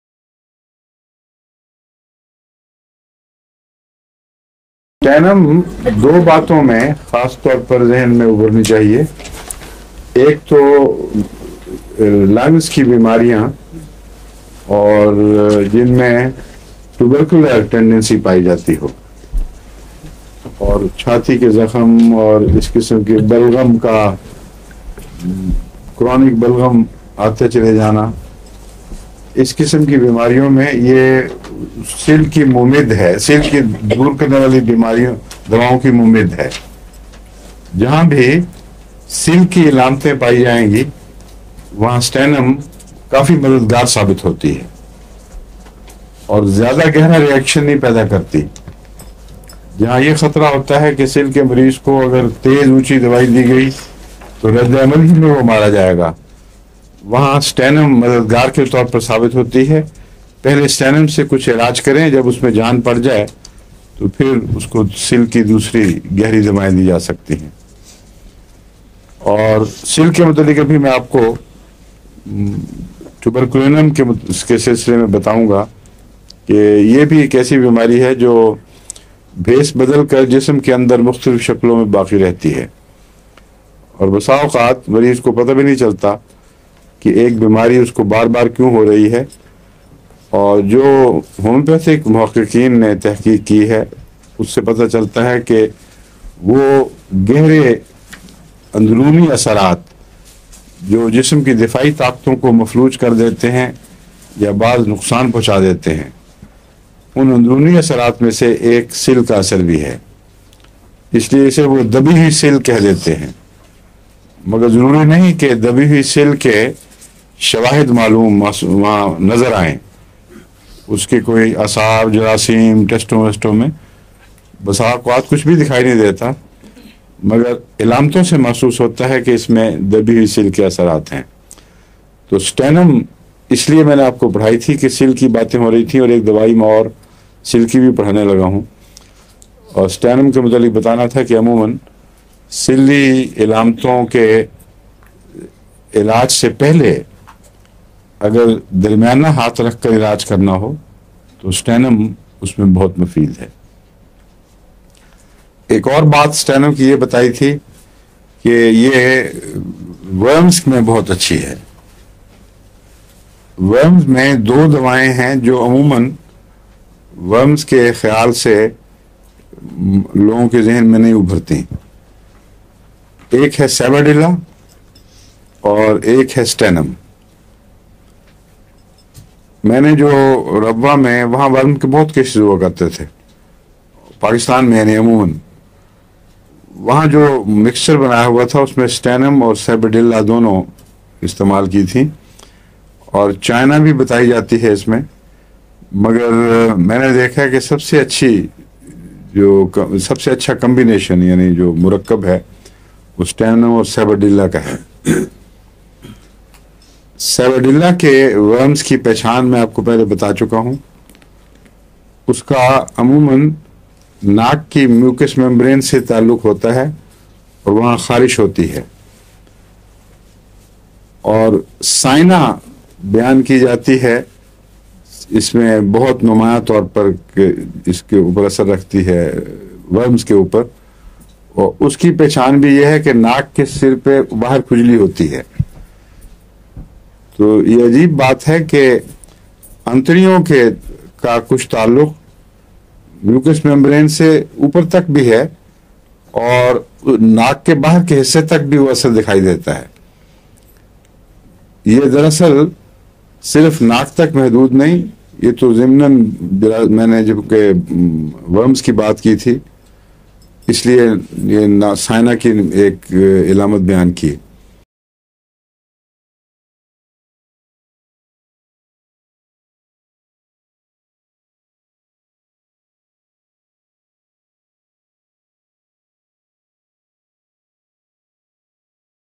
Useful in intestinal worms and lungs diseases. | H.M Tahir Ahmad | Stannum sabadilla cina
Peat ky kiron mean mufeed.

Homeopathy lecture by Hazrat Mirza Tahir Ahmad.

Class ...23/01/1995

Video Chapter
00:00 title of the video.

These classes is from 1994 to 1997 ,that's why video and audio quality is not too good.
But I did some work on it...try to increase volume and reduce some noise.
Good handsfree also can give you high volume quality.

Needs your Prayers.
آپ کی دعاؤں کا طلب گار۔ جزاک اللّٰہ